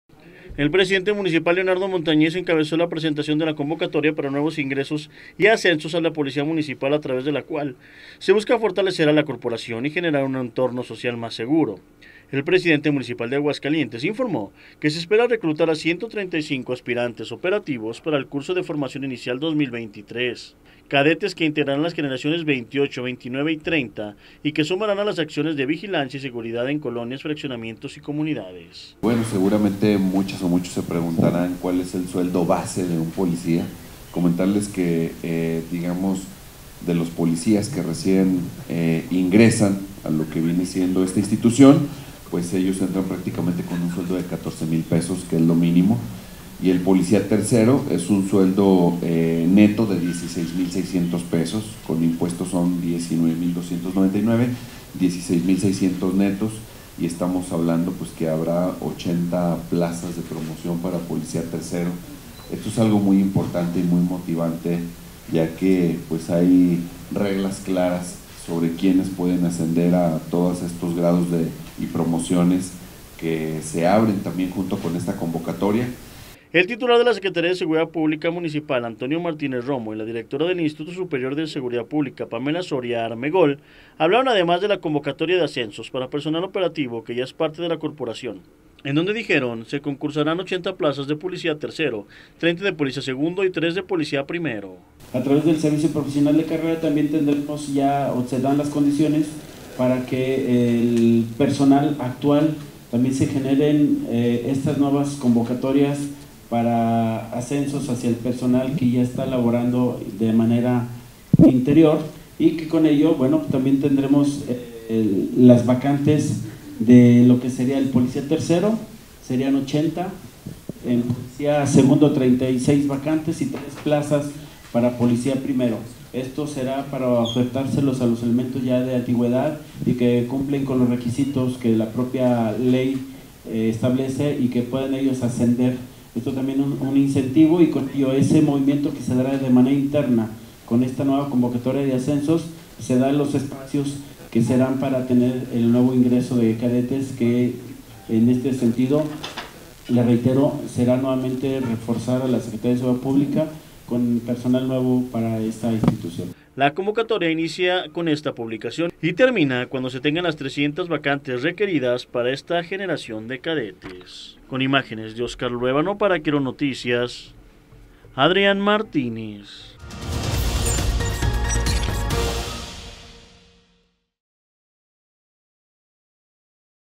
Nueva convocatoria en la Secretaría de Seguridad Pública Municipal
Abren convocatoria para 300 aspirantes a policías municipales, además habrá una convocatoria interna para asensos de grados.